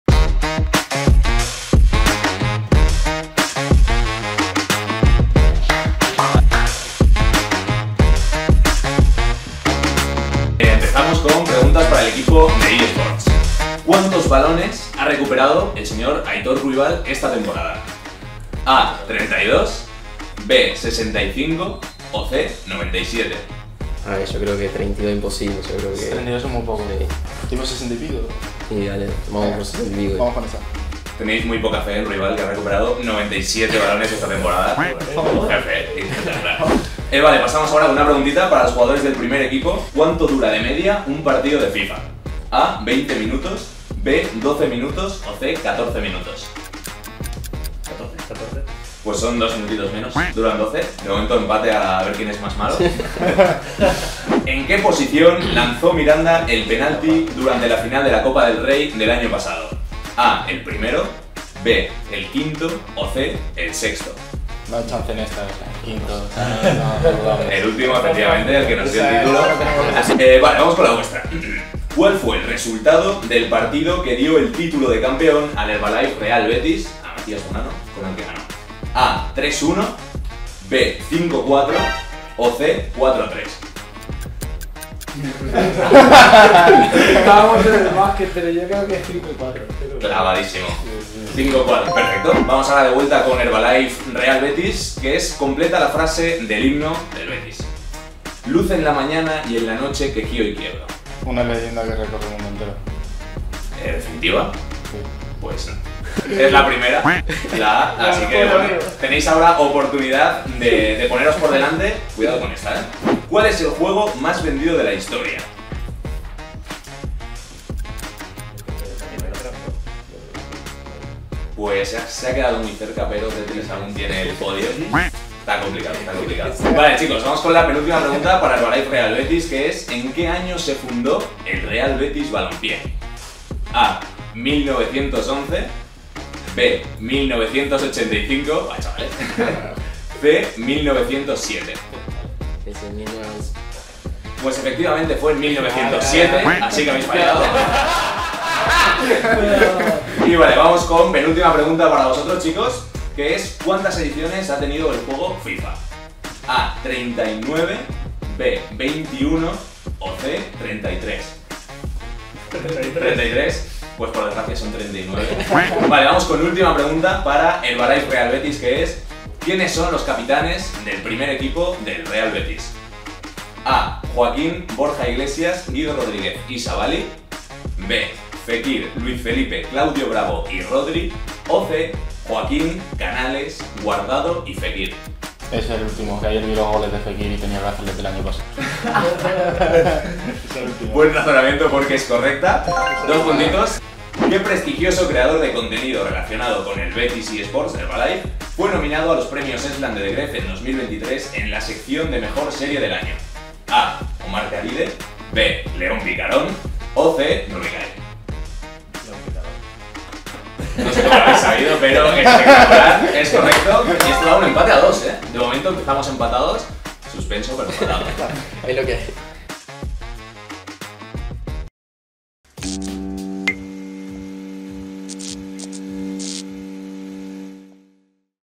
BORJA IGLESIAS y AITOR RUIBAL se enfrentan a los jugadores del Herbalife Real Betis 🎮🔥 | CHALLENGE
BORJA IGLESIAS y AITOR RUIBAL se enfrentan a MATÍAS BONANNO e IVÁN PRIETO, jugadores de nuestro equipo de eSports en este divertido reto de Herbalife.